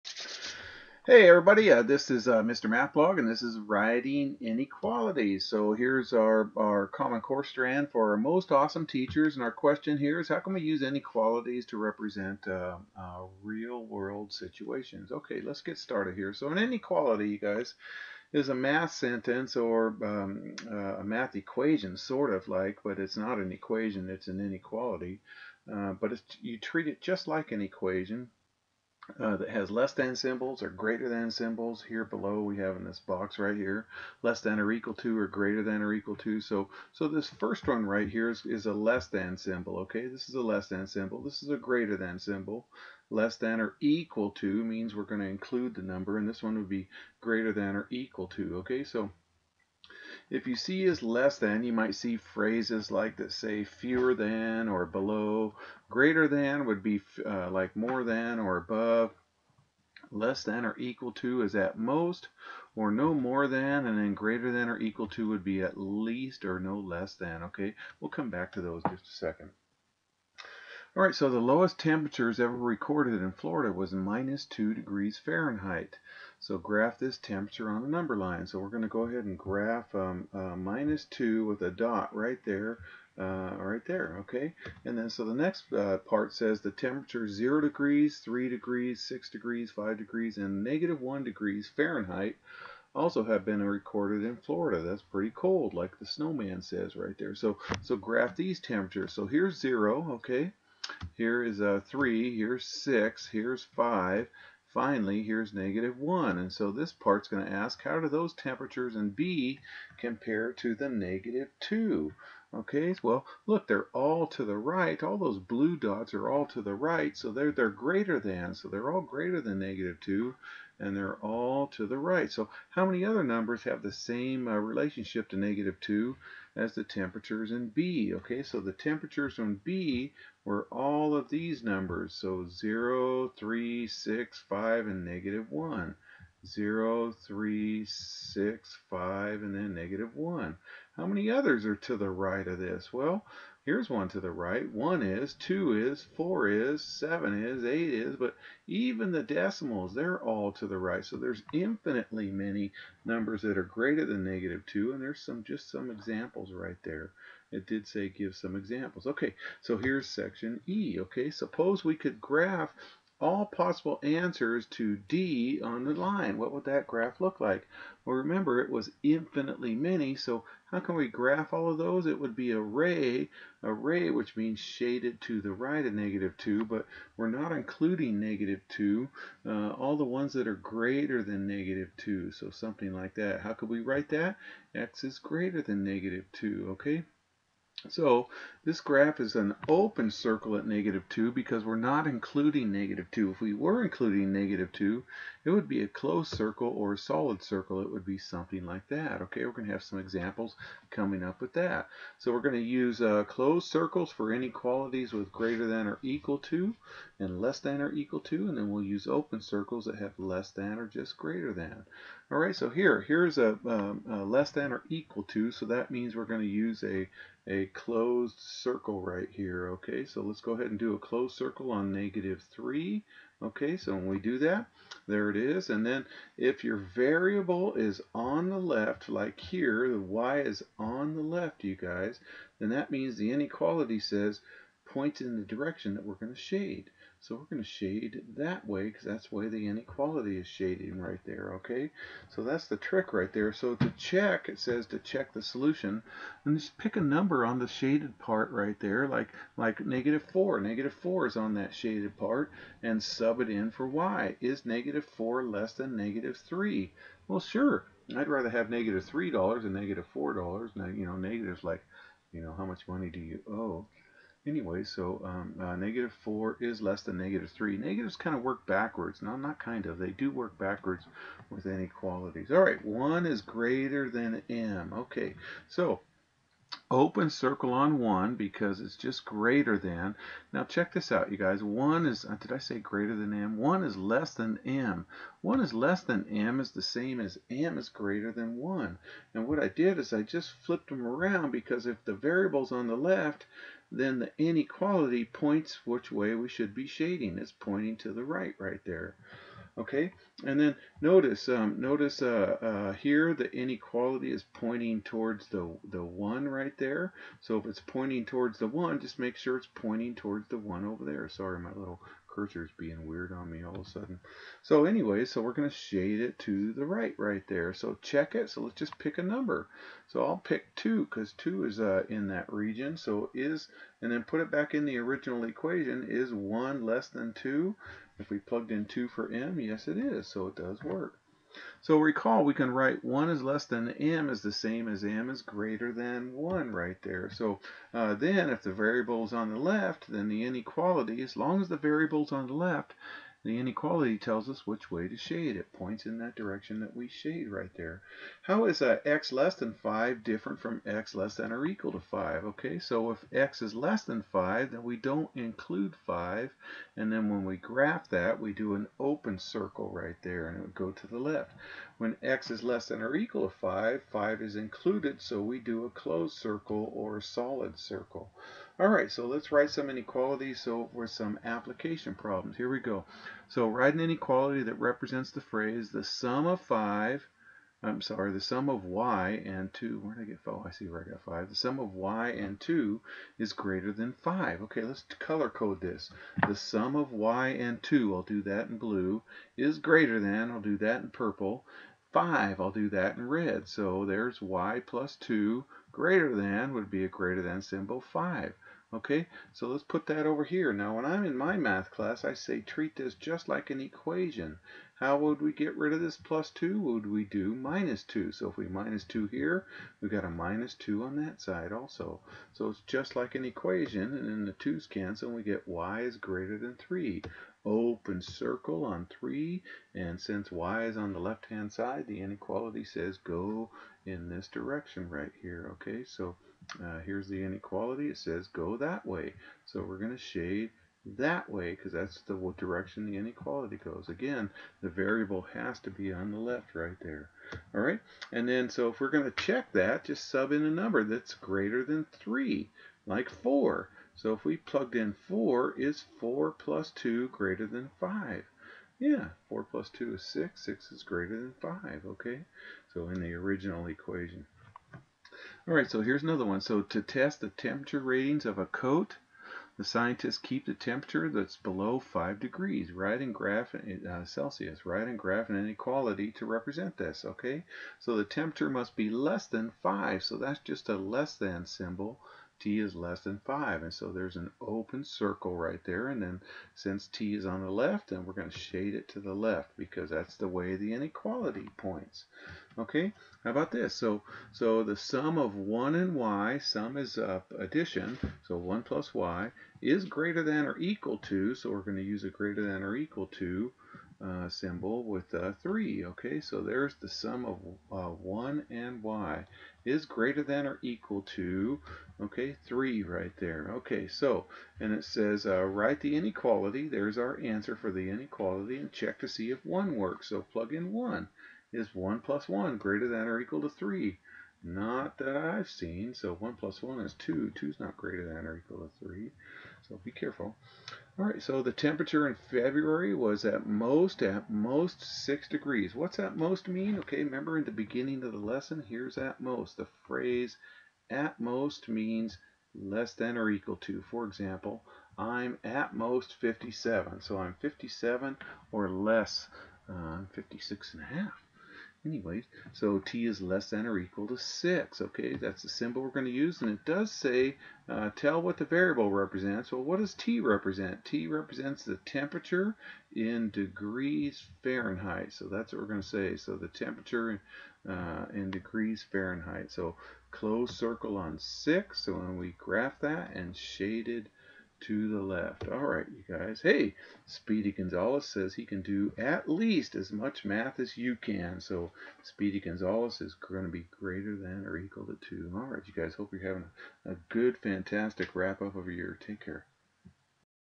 Writing Inequalities - Lesson 11.4
This lesson shows us how to write inequalities, given phrases, and how to graph these on a number line. Remember, open circles are for "greater than or less than." Closed circles are for "greater than or equal to, or less than or equal to.